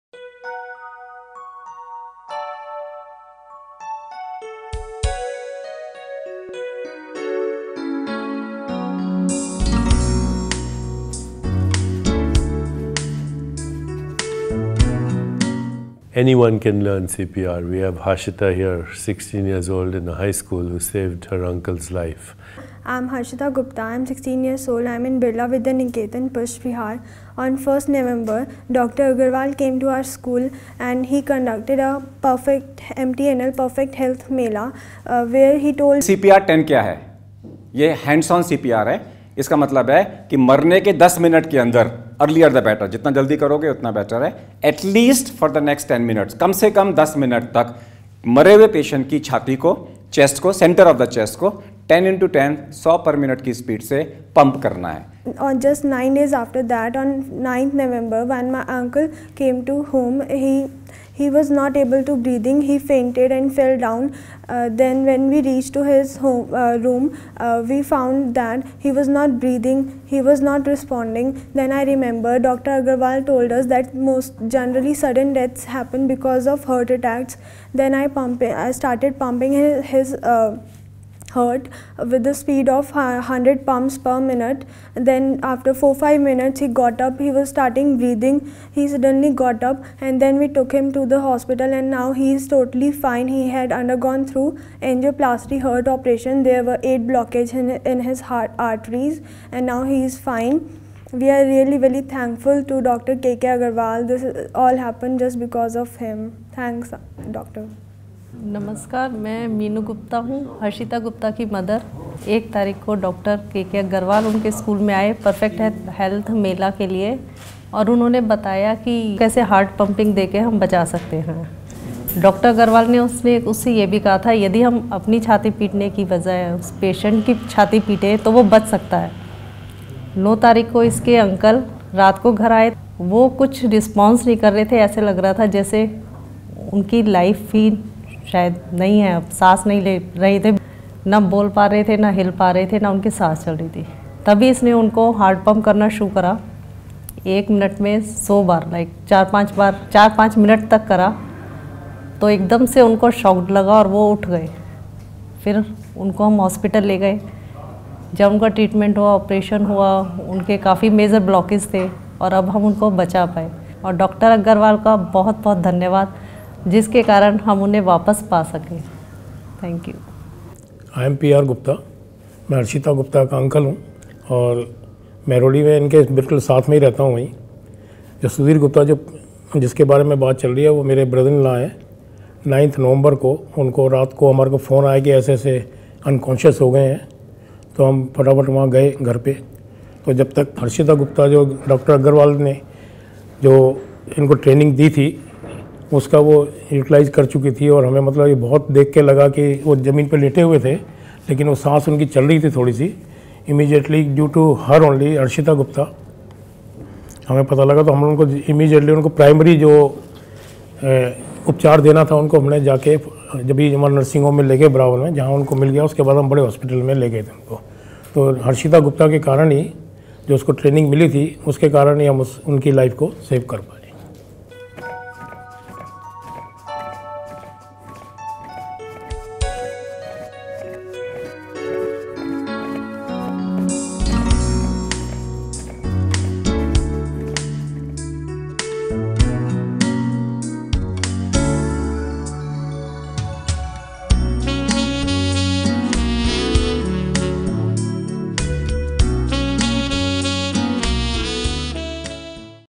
School girl does successful hands only CPR 10
CPR 10 Is based on the formula of ten created by Padmashri Awardee Dr K K Aggarwal " within ten minuets  death, earlier the better, for the next atlest ten minutes, compress the center of the chest of a patient with cardiac arrest with a speed of 100 ( 10×10) per minute